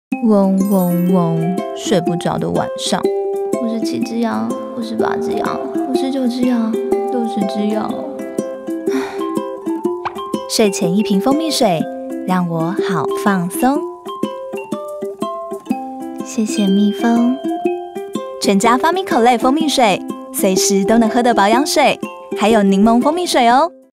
【良品再發現】睡不著的晚上 ::全家::FamilyMart Collection蜂蜜水、檸檬蜂蜜水
【FamilyMart Collection蜂蜜水小百科！隨時都能喝的保養水】
蜜蜂工坊與全家聯名
推出從早到晚都適合飲用的珍貴蜂蜜水呦🐝
快來看看蜂蜜小百科👀
🐝蜂蜜可以幫助消化、調整體質、補充元氣
🕚睡前翻來覆去難以入眠，該怎麼辦才好呢？😴
👉喝一瓶蜂蜜水讓你好放鬆！

🌟蜂蜜水*‧°★*
🌟檸檬蜂蜜水*‧°★*